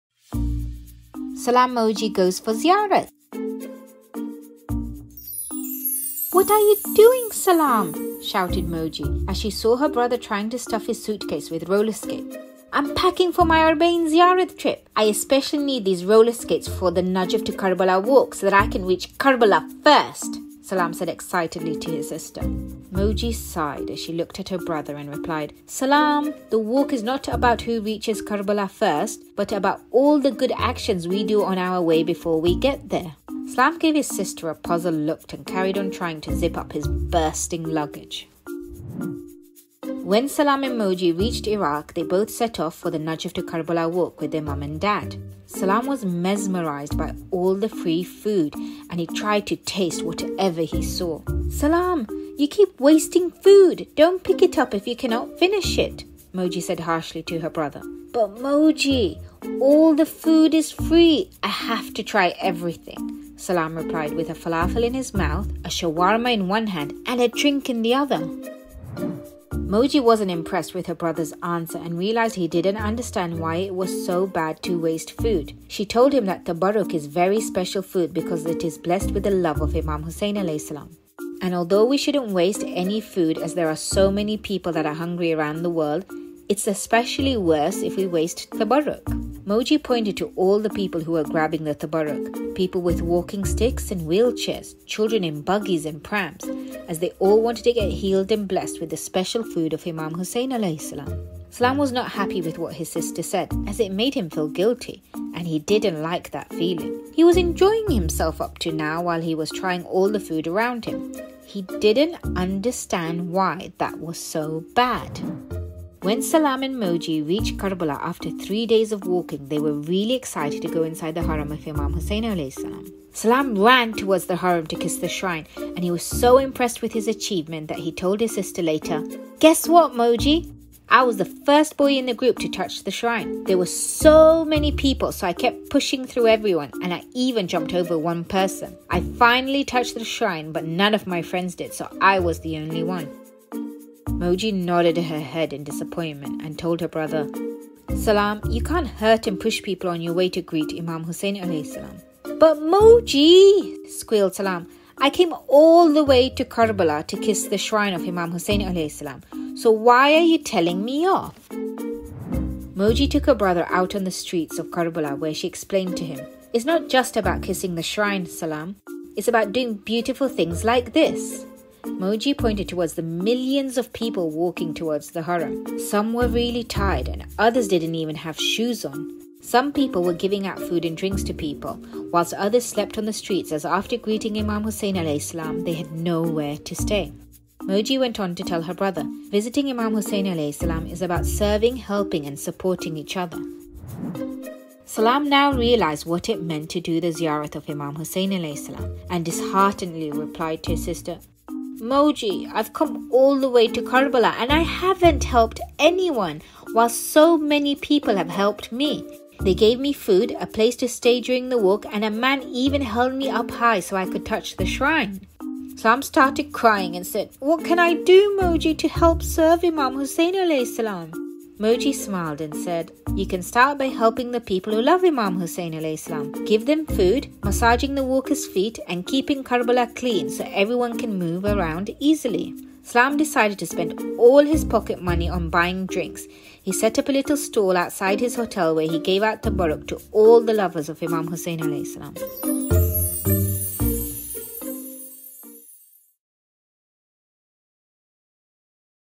Salamoji goes for Ziyarat
Salam thoroughly enjoys his Ziyarat trip, especially all the free food and places to rest.  His doting sister Moji explains to him to look beyond wasting food and taking advantage of kindness and describes the etiquette of visiting Imam Hussain (a.s) Haram.